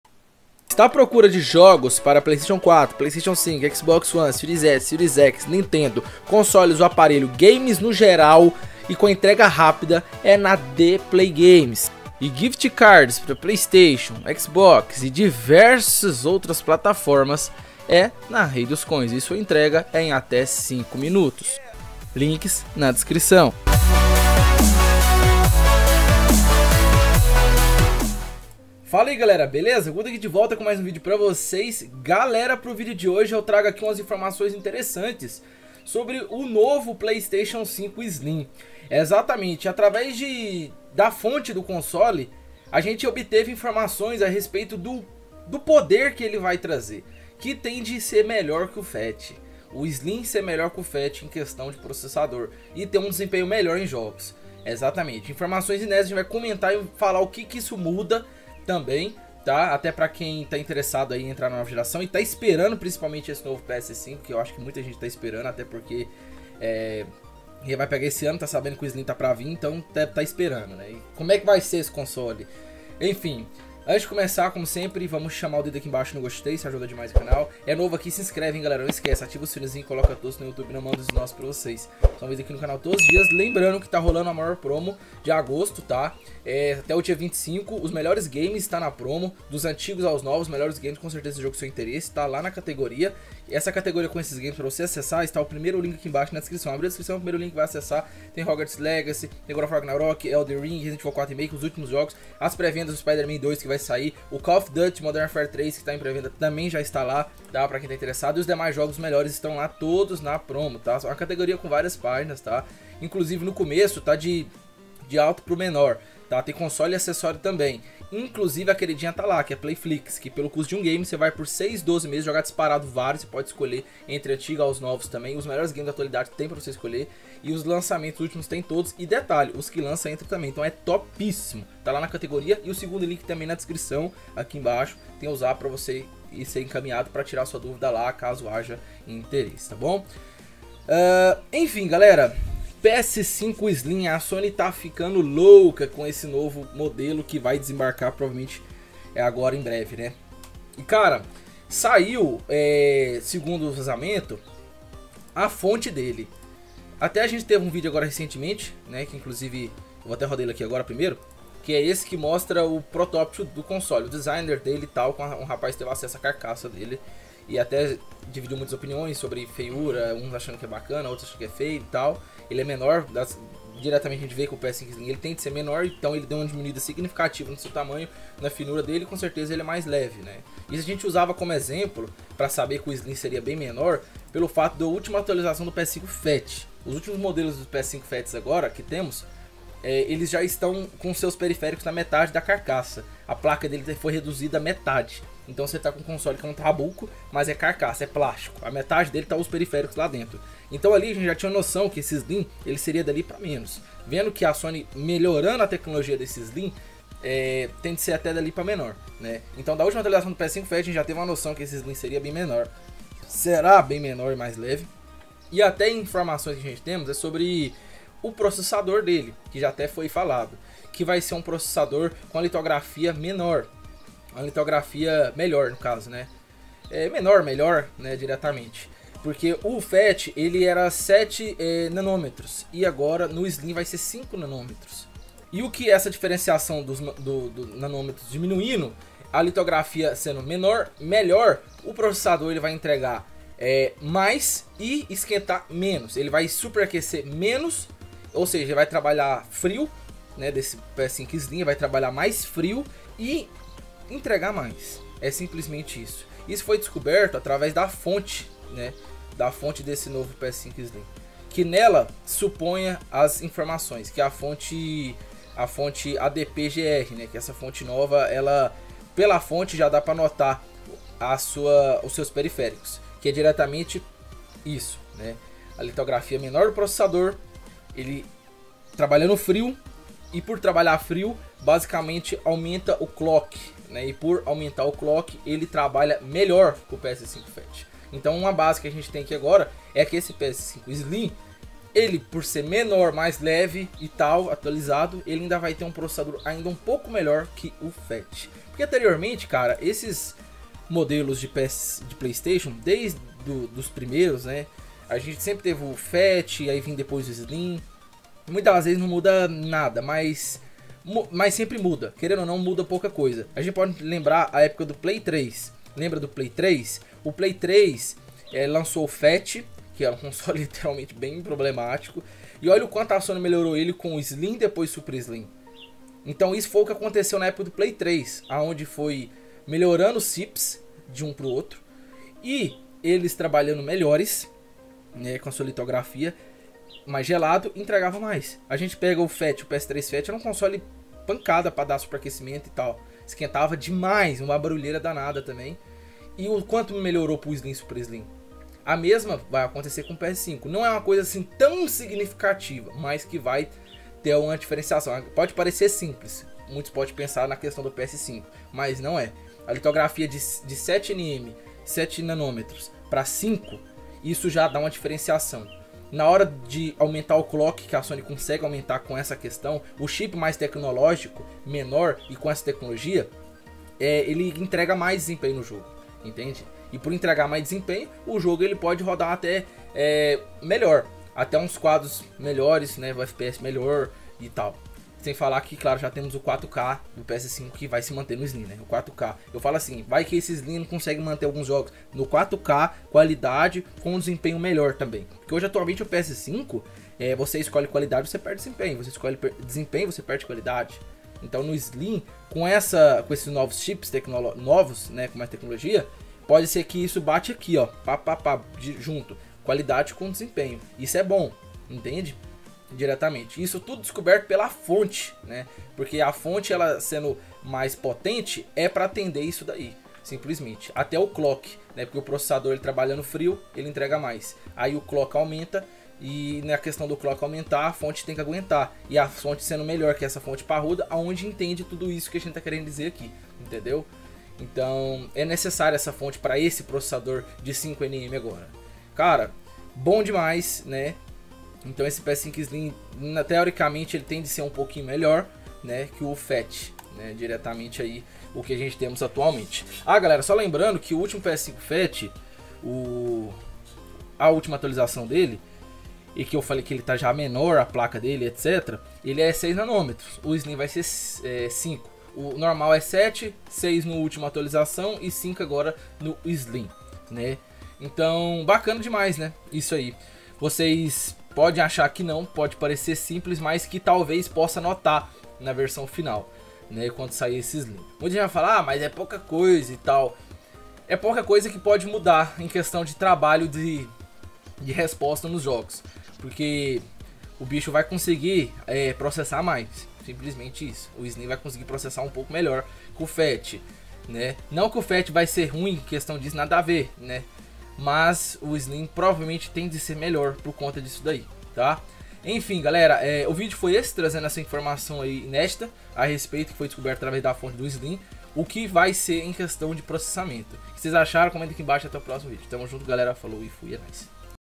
A SONY FICOU LOUCA!! NOVO PS5 SLIM E MAIS PODEROSO QUE O PS5 FAT ATUAL !! AGORA TUDO EM 4K 60FPS
No vídeo falo de poder do novo ps5 slim

Wh4tsApp ZAP para duvidas e Adquirir ( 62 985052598 ) Chama no Zap e tire suas duvidas🎮

vídeos novos todos os dias nesse canal